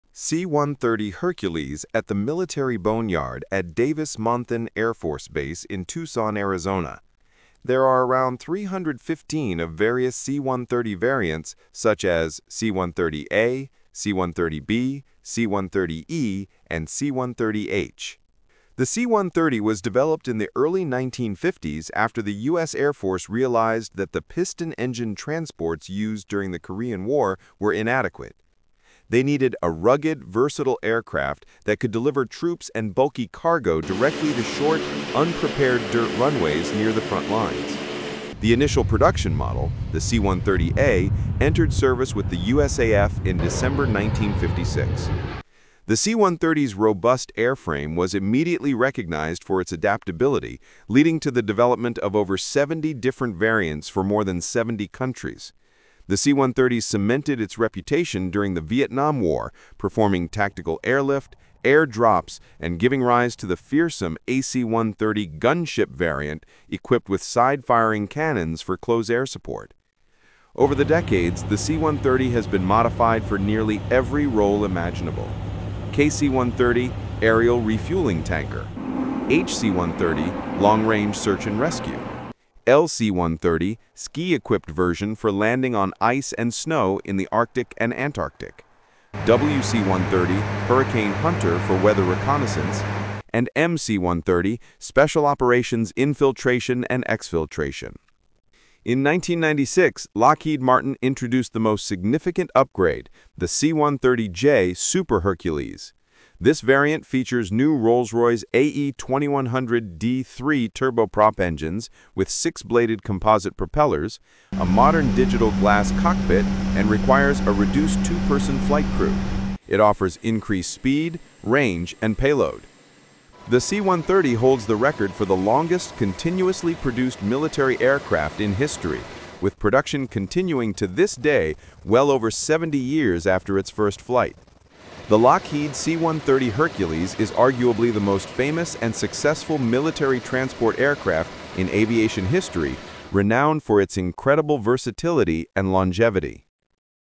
C-130 Hercules at the Military Boneyard
C-130 Hercules at the Military Boneyard at Davis-Monthan Air Force Base in Tucson, Arizona.
There are around 315 of various C-130 variants (such as C-130A, C-130B, C-130E, and C-130H).
The C-130 was developed in the early 1950s after the U.S. Air Force realized that the piston-engine transports used during the Korean War were inadequate. They needed a rugged, versatile aircraft that could deliver troops and bulky cargo directly to short, unprepared dirt runways near the front lines. The initial production model, the C-130A, entered service with the USAF in December 1956.
The C-130’s robust airframe was immediately recognized for its adaptability, leading to the development of over 70 different variants for more than 70 countries. The C-130 cemented its reputation during the Vietnam War, performing tactical airlift, airdrops, and giving rise to the fearsome AC-130 Gunship variant, equipped with side-firing cannons for close air support. Over the decades, the C-130 has been modified for nearly every role imaginable:

KC-130: Aerial refueling tanker. HC-130: Long-range search and rescue. LC-130: Ski-equipped version for landing on ice and snow in the Arctic and Antarctic. WC-130: "Hurricane Hunter" for weather reconnaissance. MC-130: Special operations infiltration and exfiltration.

In 1996, Lockheed Martin introduced the most significant upgrade: the C-130J Super Hercules. This variant features new Rolls-Royce AE 2100D3 turboprop engines with six-bladed composite propellers, a modern digital glass cockpit, and requires a reduced two-person flight crew. It offers increased speed, range, and payload. The C-130 holds the record for the longest continuously produced military aircraft in history, with production continuing to this day, well over 70 years after its first flight.
The Lockheed C-130 Hercules is arguably the most famous and successful military transport aircraft in aviation history, renowned for its incredible versatility and longevity.